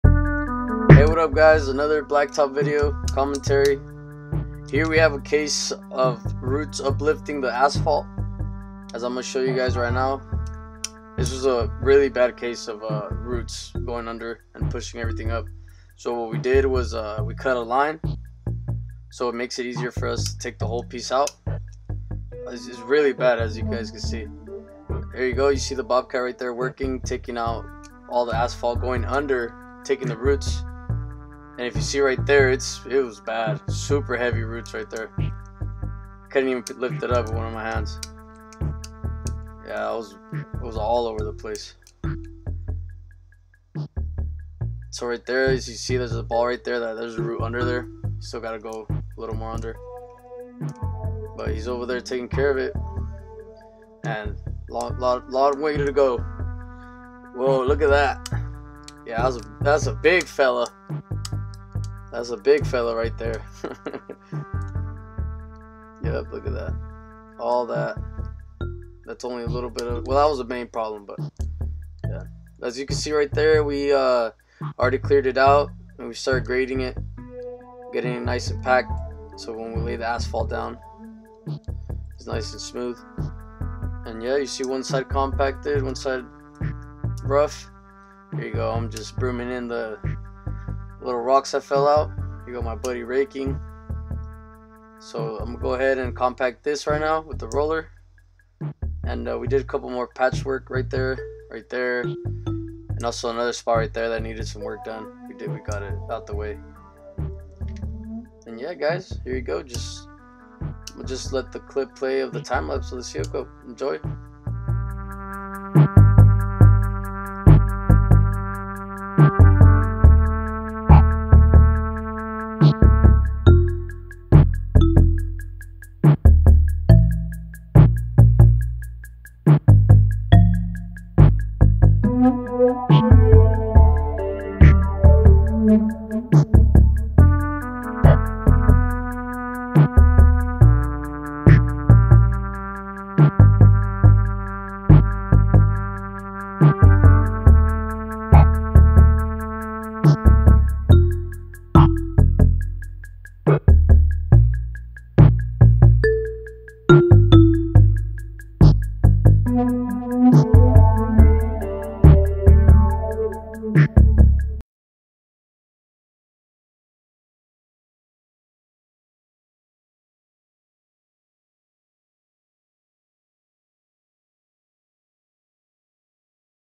root parkinglot problem
bad root problem destroying the parking lot making it unsafe. we went and removed the problem adding new asphalt, then sealed it all.